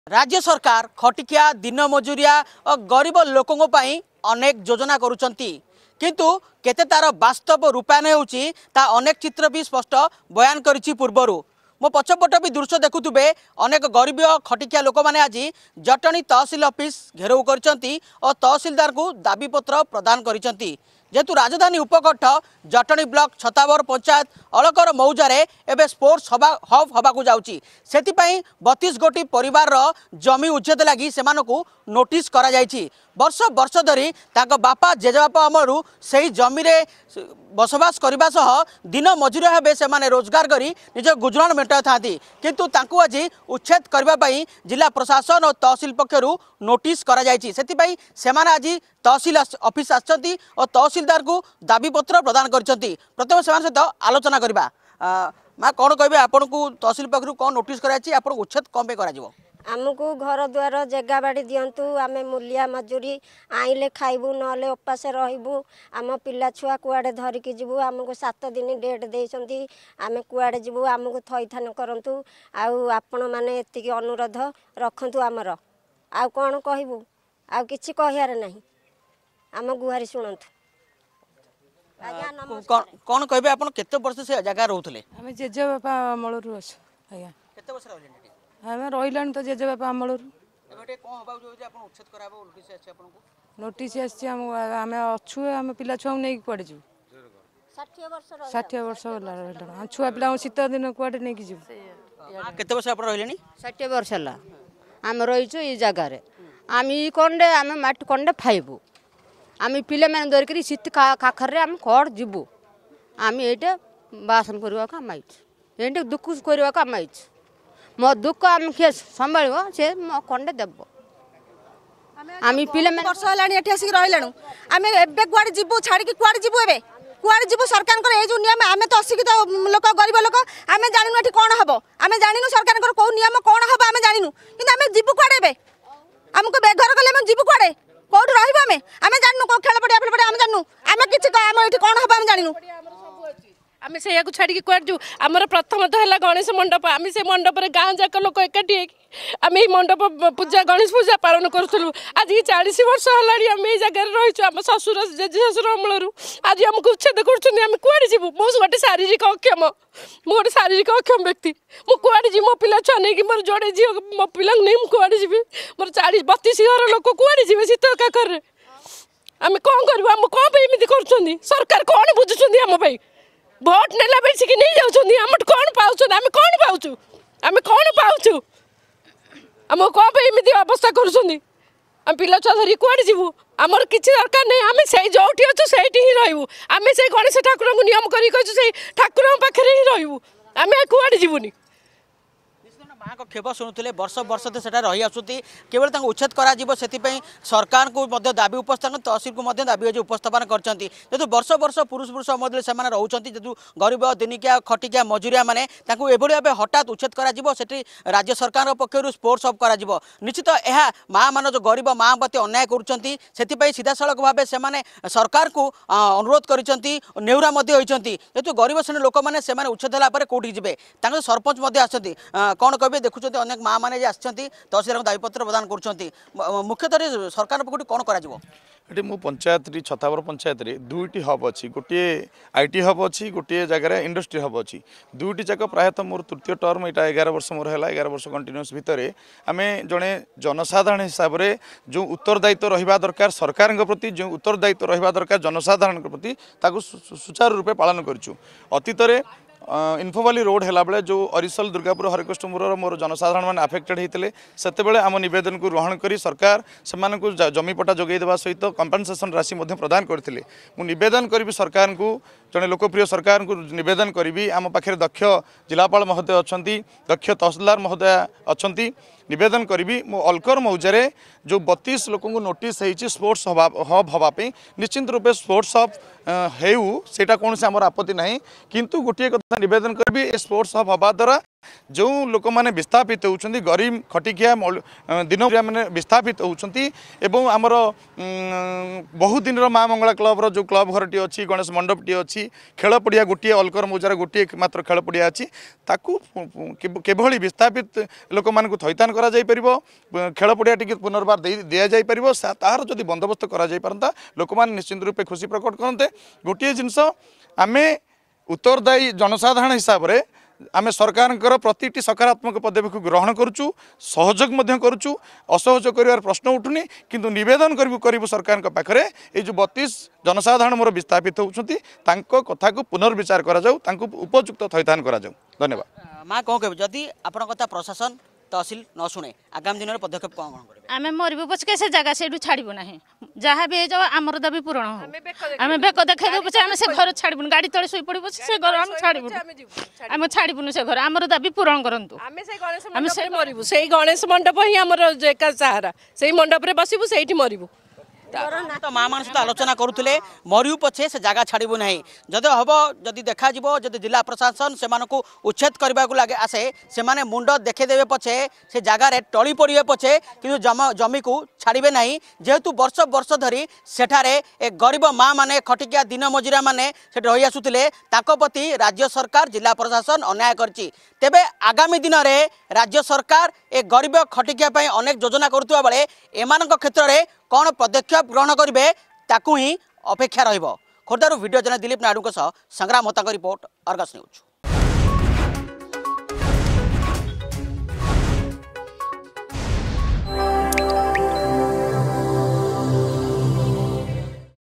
ମରିବୁ ପଛେ ଇଞ୍ଚେ ଜାଗା ଦେବୁନାହିଁ !
ଗରିବ ଖଟିଖିଆ ଓ ଦିନମଜୁରିଆ ମାନଙ୍କୁ ରାଜ୍ୟ ସରକାରଙ୍କର କୁଠାରଘାତ । ବର୍ଷ ବର୍ଷ ଅମଳରୁ ରହିଆସୁଥିବା ଜାଗାକୁ ଖାଲି କରିବା ପାଇଁ ତହସିଲର ନୋଟିସ । ଜଟଣୀ ତହସିଲ ଅଧିନ ଅଳକର ଓ ଗୌଡ଼କାଷିପୁର ମୌଜାରେ ୩୨ ଘର କୁ ଉଚ୍ଛେଦ ପାଇଁ ନୋଟିସ ହୋଇଛି. ।ଏହାକୁ ବିରୋଧ କରି ଆଜି ମା' ମାନେ ତହସିଲଦାରଙ୍କ ଦ୍ବାରସ୍ଥ ହୋଇଛନ୍ତି